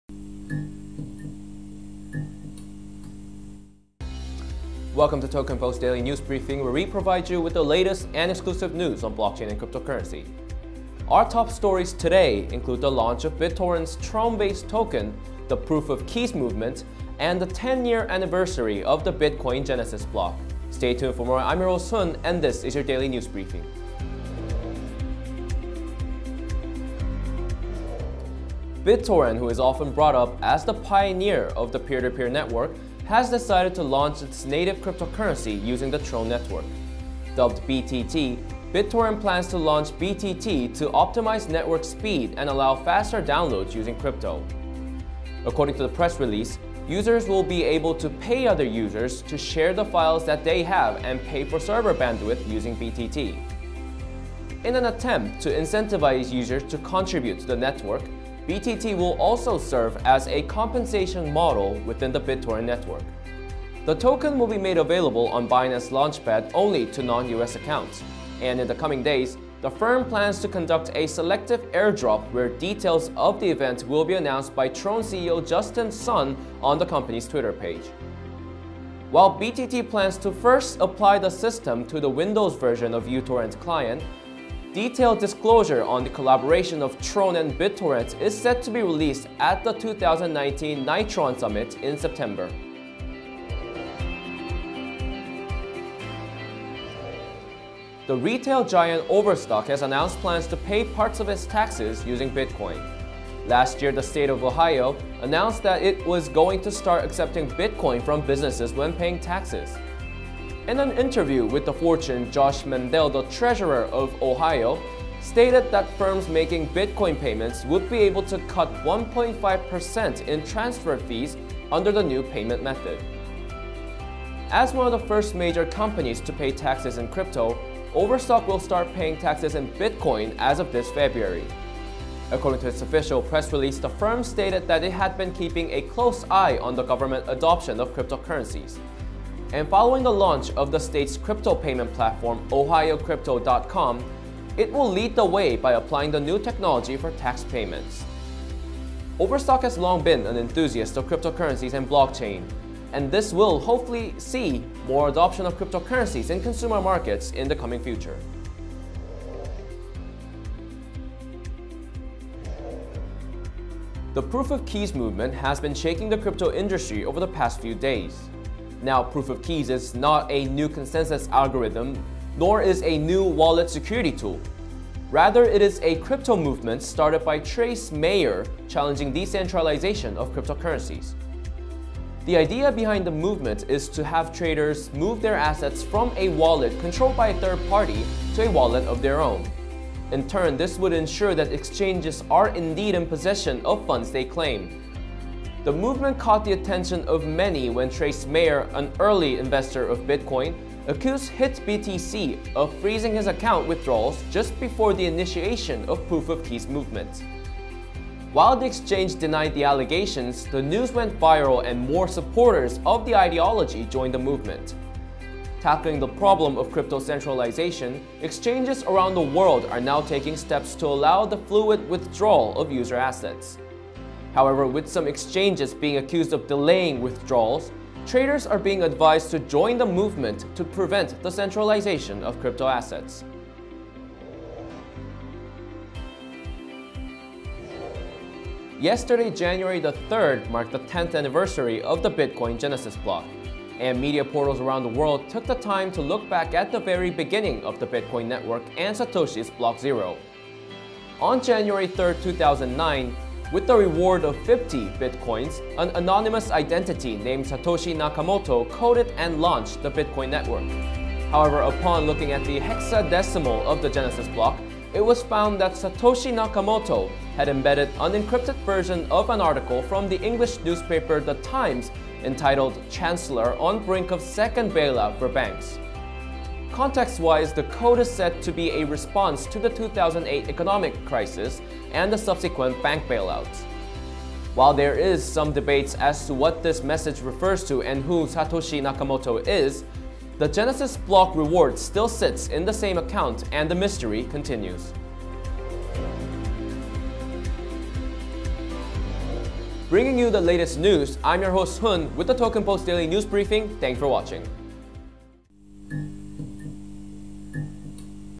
[TP Daily News] 20190104 BITTORRENT LAUNCHES TRON-BASED TOKEN / PROOF OF KEYS / BITCOIN TURNS 10
[00:28] BITTORRENT LAUNCHES TRON BASED TOKEN

[01:37] OVERSTOCK STARTS  PAYING TAXES WITH BTC

[02:43] PROOF-OF-KEYS, ‘BANK RUN’ AGAINST EXCHANGES’

[03:58] BITCOIN TURNS 10, SATOSHI’S ‘BLOCK 0’’